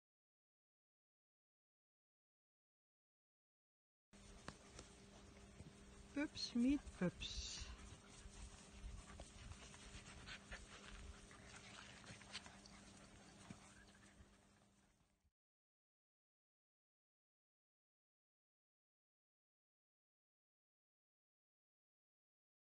Emmerpups DRG
Wij zijn met z'n allen in een emmer voor de deur gezet toen we nog erg klein waren.
De mensen hier hebben ons gewassen en liefdevol verzorgd.
We zijn ook naar de Dierenarts geweest.
Die zei dat we allemaal een beetje mager, maar wel gezond waren.